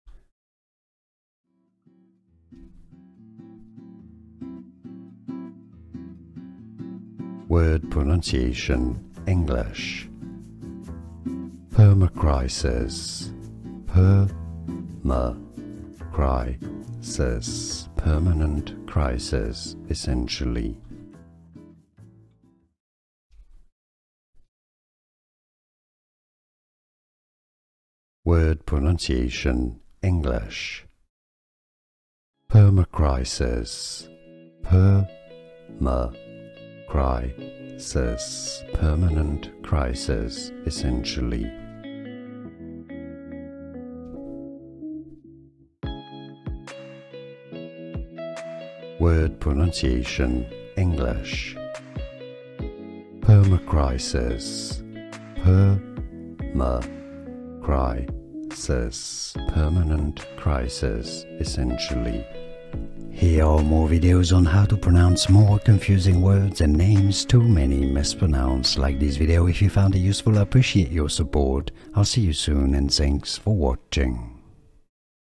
How to Pronounce Permacrisis
This video shows you How to Pronounce Permacrisis, pronunciation guide.

00:00 - Pronunciation Intro
00:16 - How to Pronounce Permacrisis

How to Pronounce Permacrisis. What does this word/name mean? Words’ meaning, dictionary definition, explanation.
Information & Source
Definition of 'permacrisis'
permacrisis
in British English
(ˈpɜːməˌkraɪsɪs IPA Pronunciation Guide )
NOUN
Word forms: plural -ses (-siːz IPA Pronunciation Guide )
an extended period of instability and insecurity, esp one resulting from a series of catastrophic events